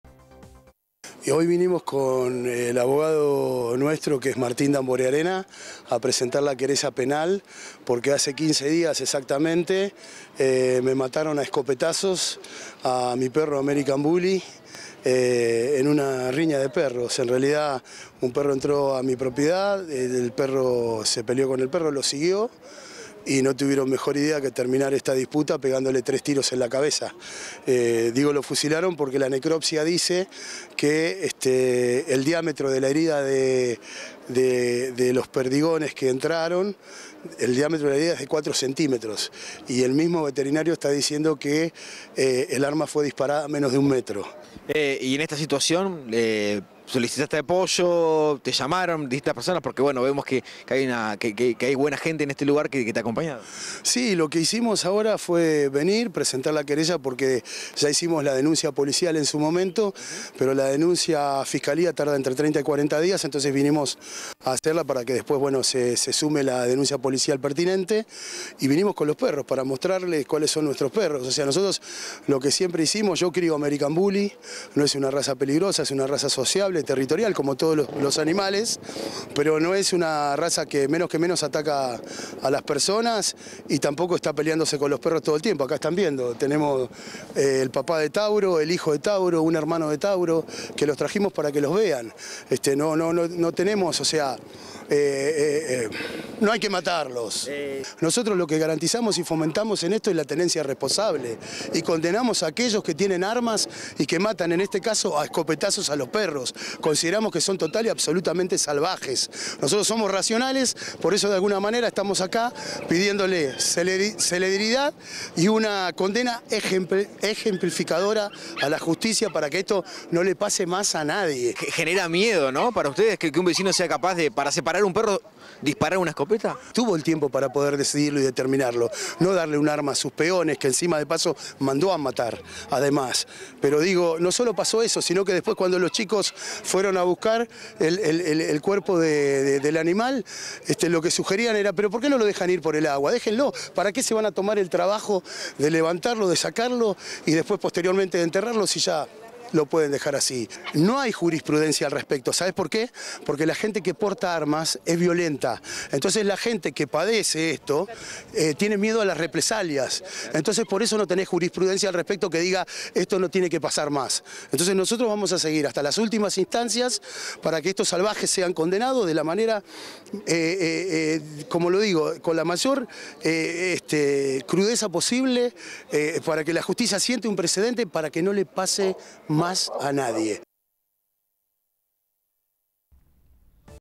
Le mataron un perro
Fernando Rodríguez, vecino de General Roca, habló en el marco de una denuncia que hizo contra vecinos  que asesinaron a uno de sus perros con disparos de escopeta. Indignado pide justicia ante este panorama de violencia y asegura que no hay motivo para fusilar un perro.